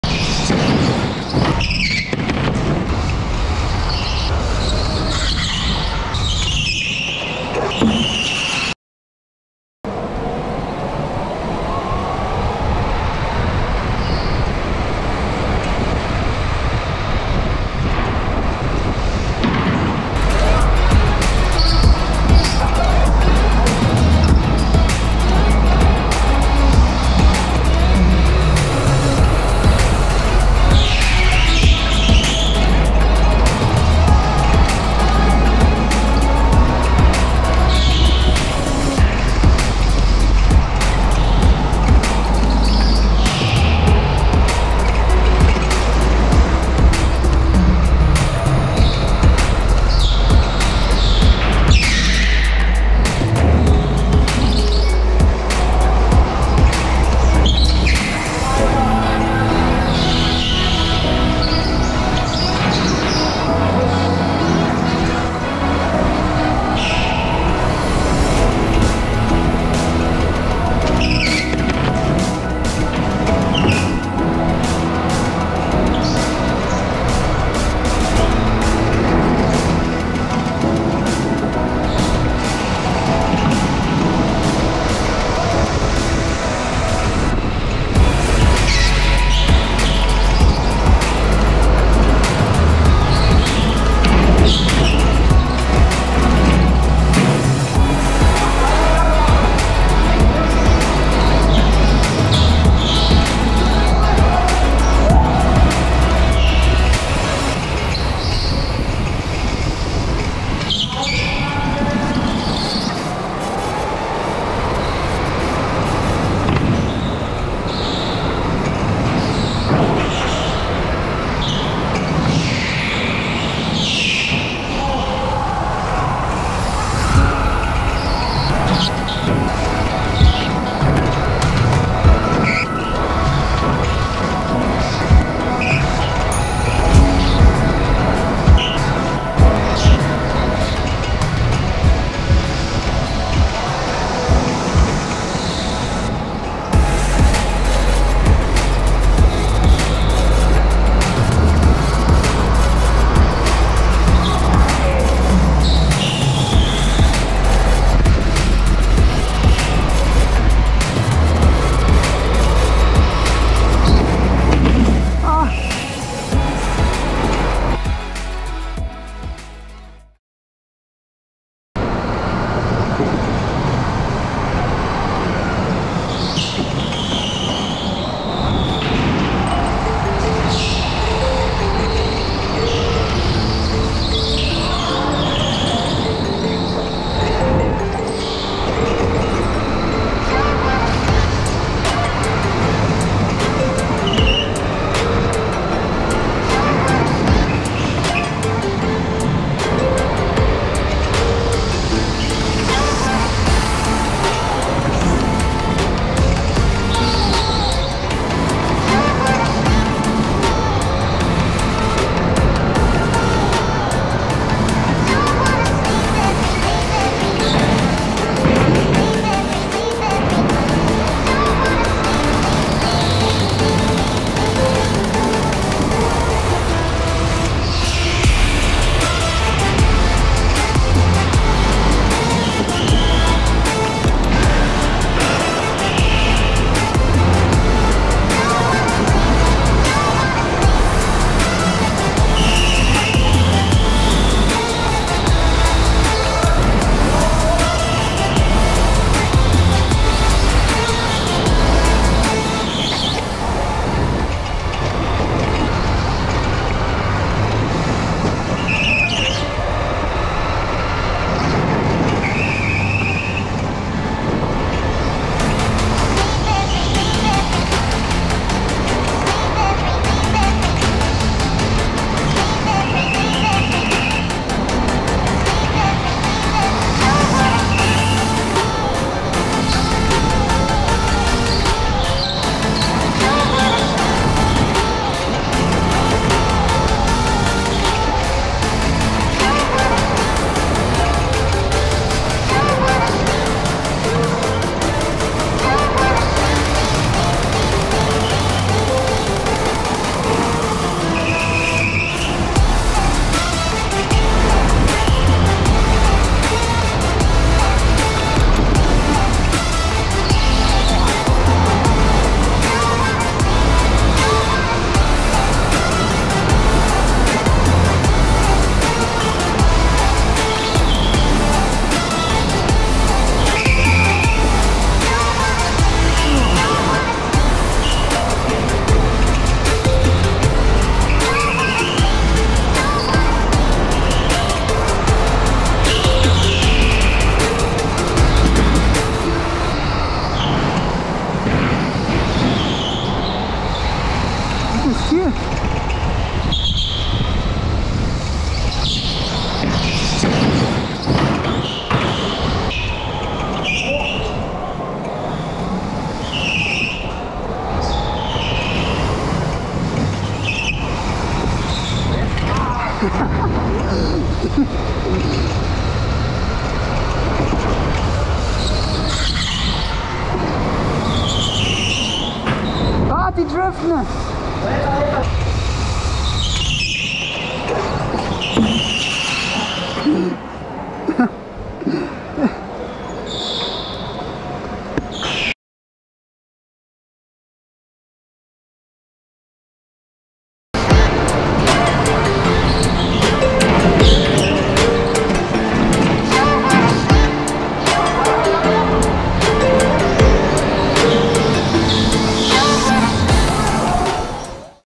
KART FAHREN | UNFALL | Kart Palast Funpark | Maxi Eichhorn
Vor ein paar Wochen war ich mit ein paar Freunden beim Kart fahren und hab mir einfach mal die GoPro an den Helm gemacht. Viel Spaß mit dem Video, lasst doch gerne ein Abo und einen Like!!!

►Mein Bike: YT Capra 27 AL Comp in Größe L bei 1,79m Körpergröße: